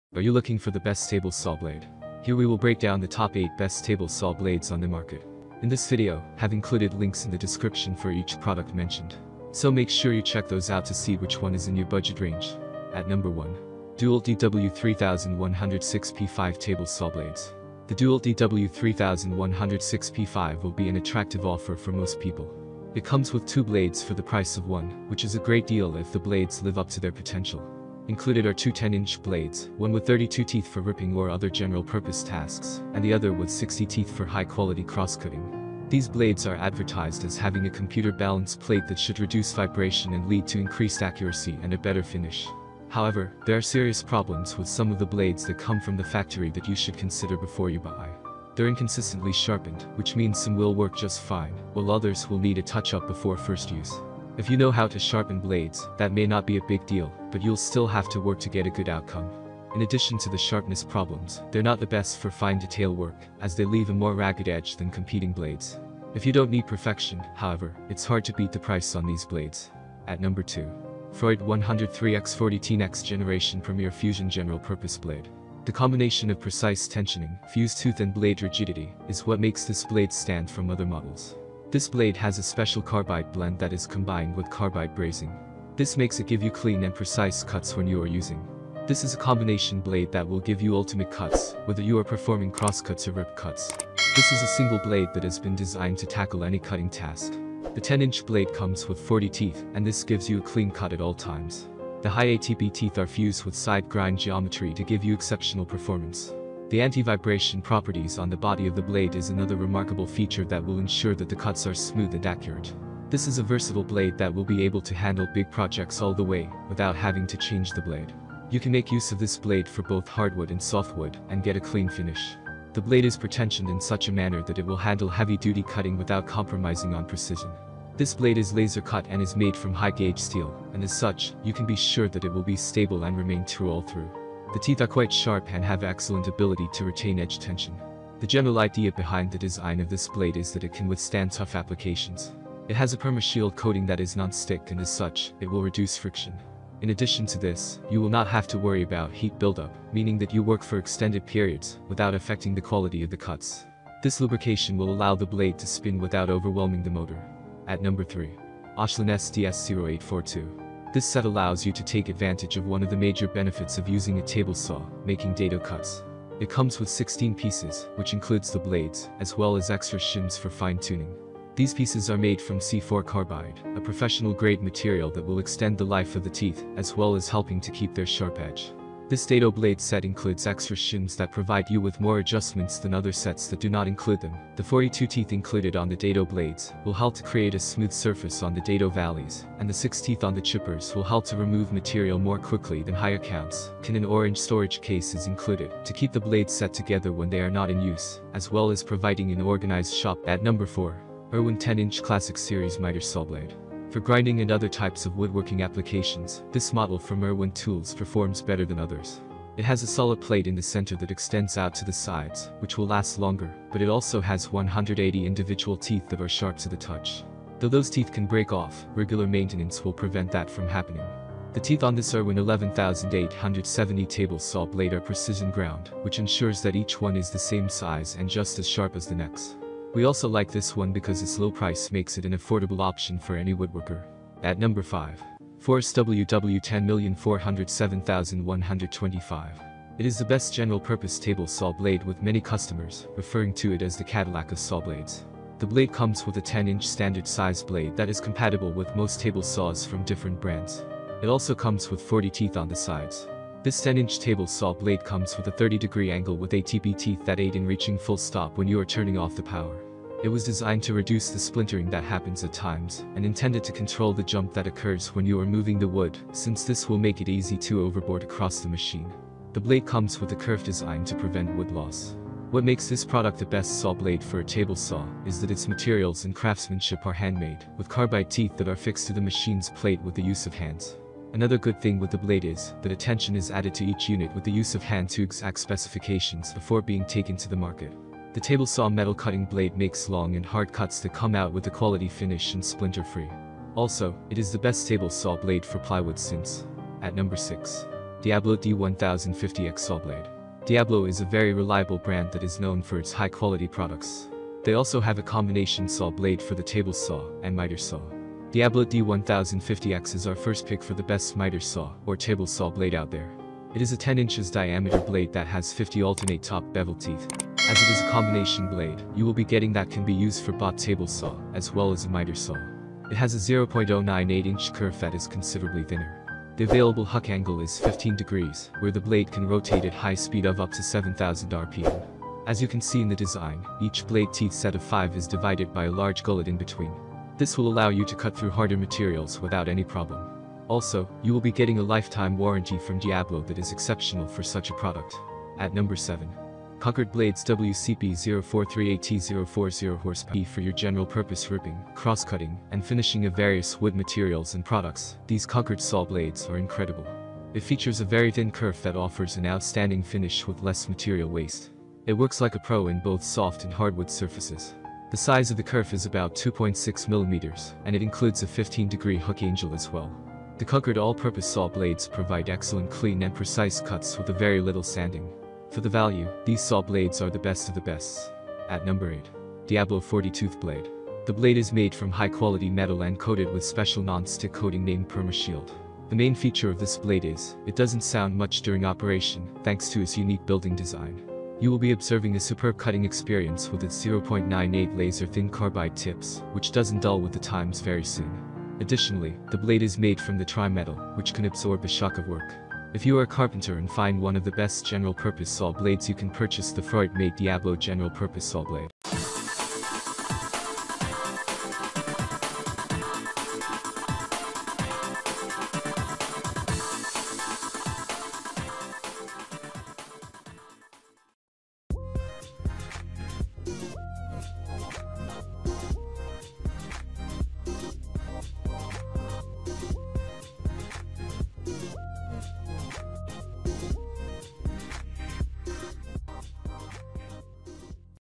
Best Table Saw Blade Reviews 2025 - Top 8 Table Saw Blades Review
Best Table Saw Blade 2025

Are you searching for the Best Table Saw Blade?

If so, you are in the right place to get essential information on Best Table Saw Blade.

In This Table Saw Blade Review Video, We Will shows you 8 top-rated Table Saw Blade buy-in 2025.  We made this list based on our personal opinion based on their price, quality, durability, brand reputation, User Feedback and other related issues.

Jump To Section:
00:00 Introduction.
00:12 Dewalt Dw3106p5 Table Saw Blades Review.
01:10 Freud  General Purpose Blade Review.
02:36 Oshlun Sds-0842 Review.
03:20 rwin 10-Inch Classic Series Miter Saw Blade Review.
03:52 Forrest Ww10407125 Review.
04:56 Diablo D1050x Saw Blade Review.
05:45 Concord Blades Wcb0438t040hp Review.
06:18 Diablo 40 Tooth Blade Review.